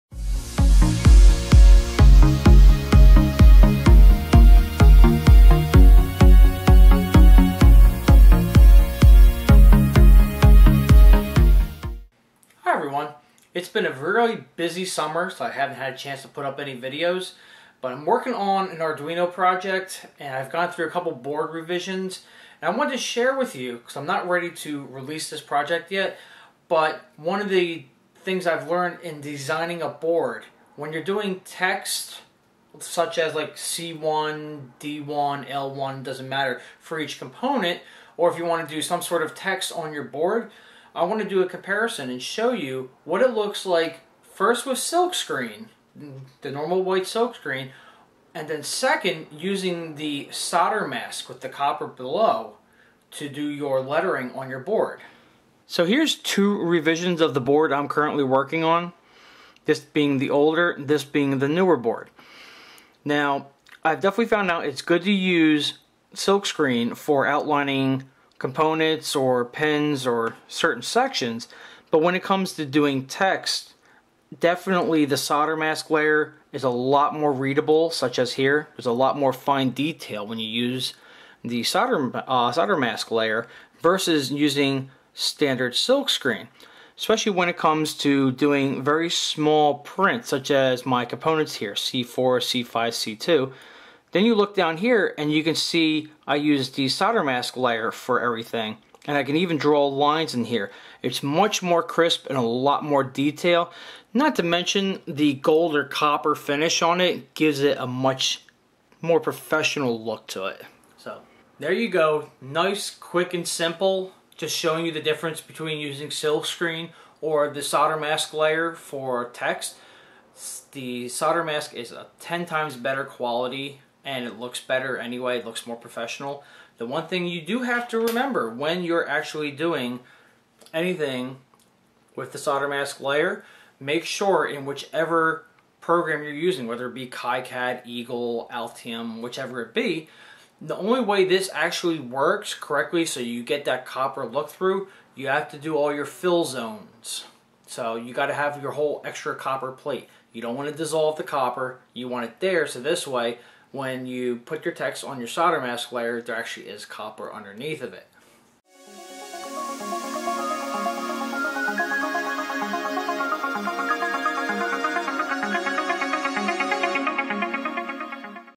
Silkscreen vs Soldermask Text On Circuit Boards
This is a quick comaprison of putting text on silkscreen versus text on the soldermask layer of your project.  The soldermask layer has much higher resolution and text can be more crisp and readable.